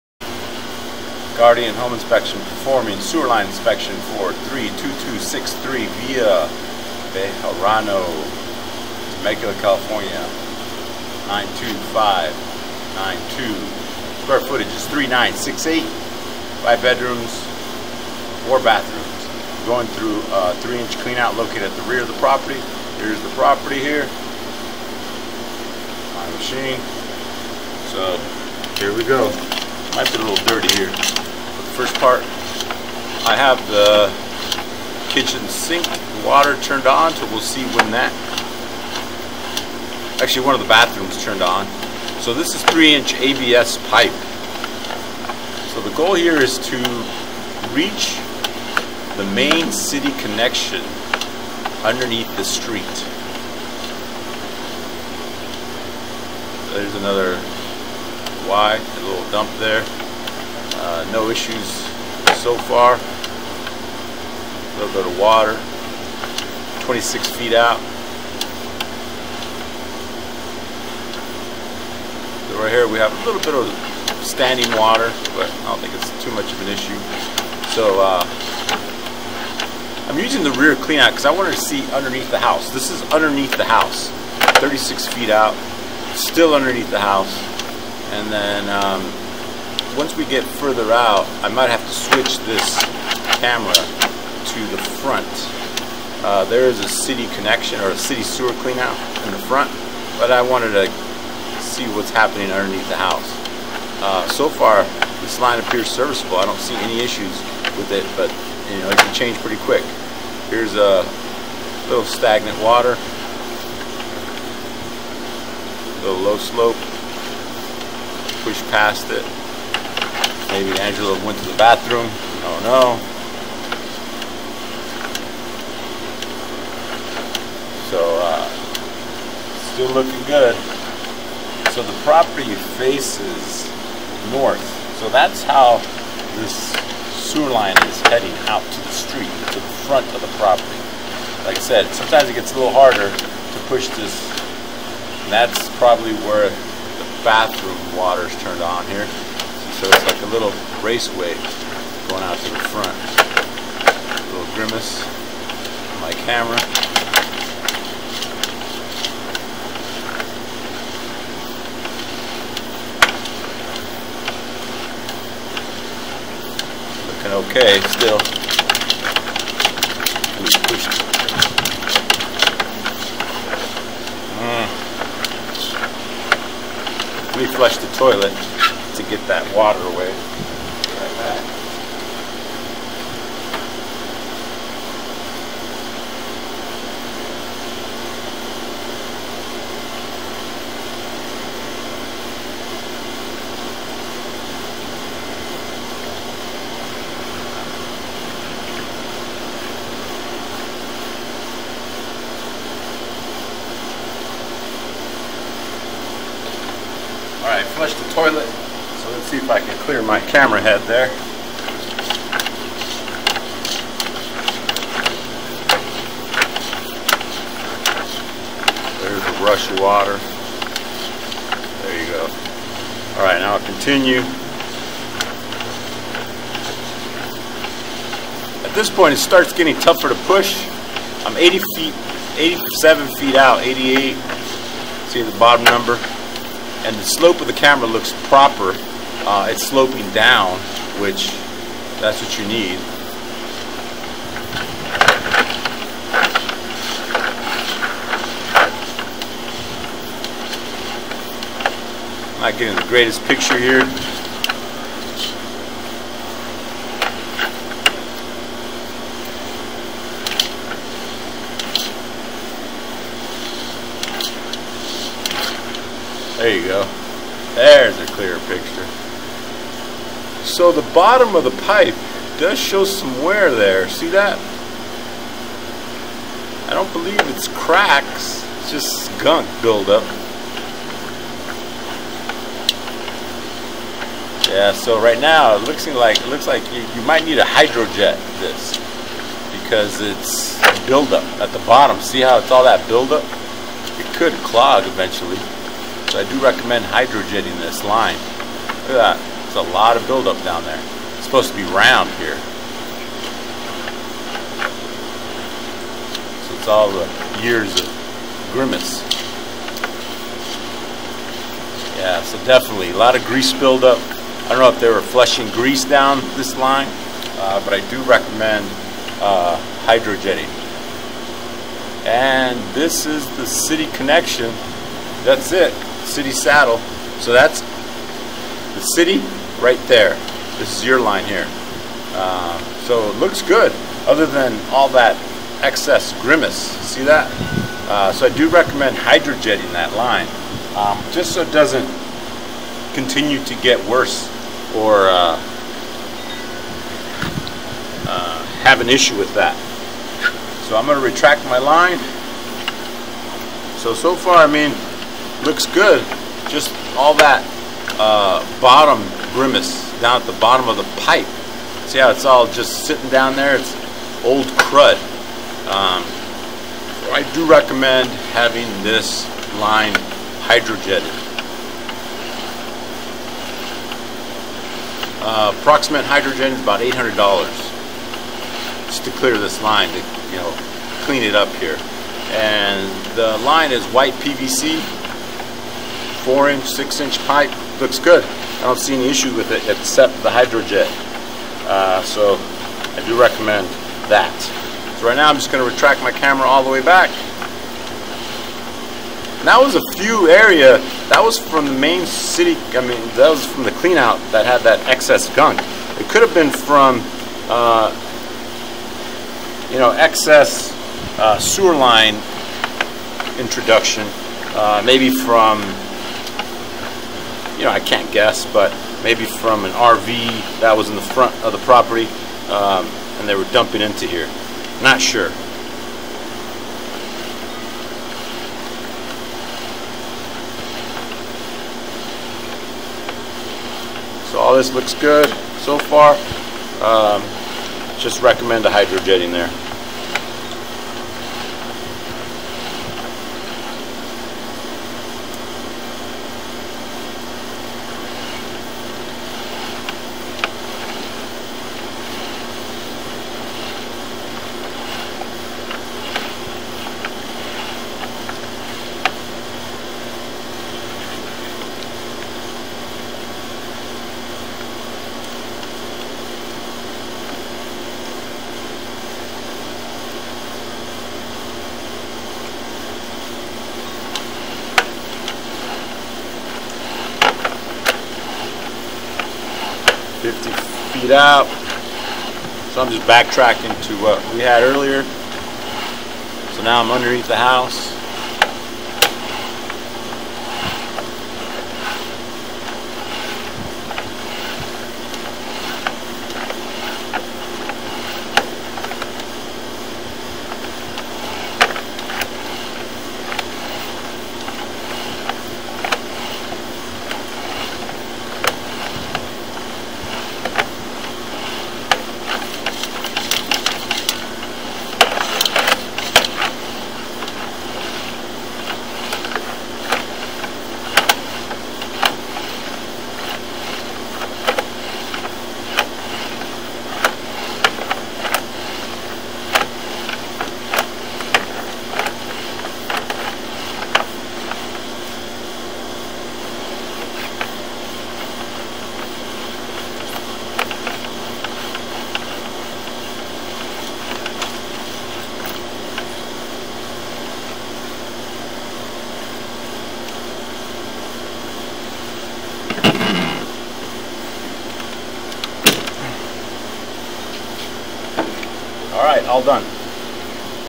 32263 Via Bejarano, Temecula Ca. 92592
Guardian Home Inspection performing diagnostic main lateral sewer line inspection for property 32262 Via Bejarano, Temecula, Ca. 92592. Inspector used rear 3" ABS clean out located at rear patio. Main sewer line transitions to 4" ABS, and then transitions to 4" PVC. The last section of main sewer pipe has grease and sludge build up that runs down length of sewer pipe all the way to the main city sewer saddle connection. The main city sewer saddle connection appears serviceable. Guardian Home Inspection recommends having main sewer line Hydro Jetted to clear the grease and sludge build up, approx price for hydro jet is $700.00 to $900.00.